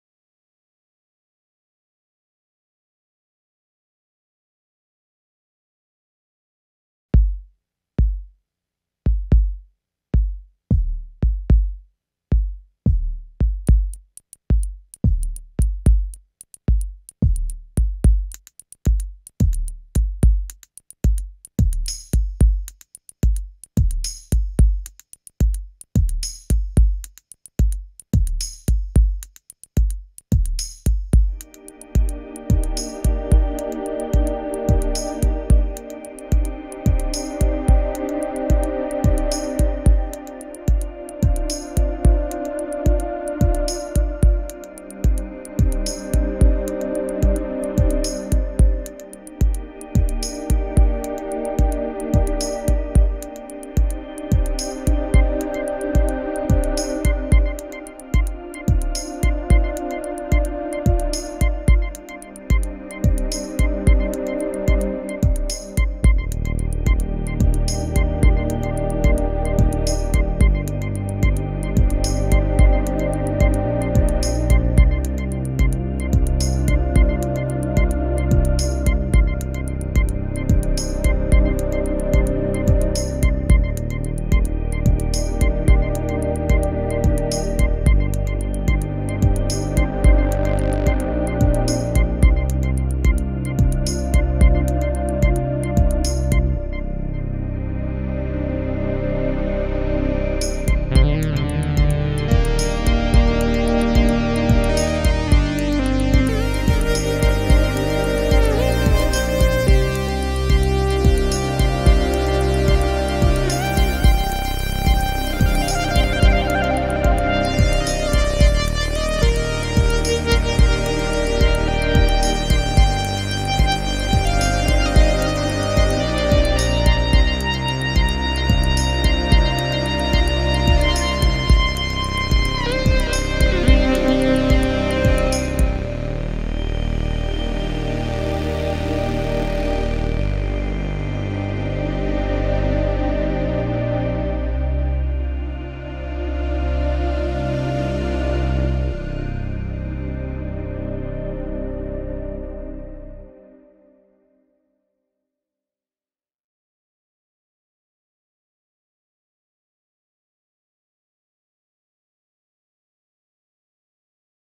Casio XW Live Looping Demo with iPad (Nave / iMS-20)
Utilizing the XW-G1's Step Sequencer and Phrase Recorder, Casio's Mike Martin creates a composition LIVE. Using a combination of the XW-G1's internal sounds as well as the iMS-20 and NAVE iPad apps.  Nothing is pre-recorded and software such as Ableton is NOT used.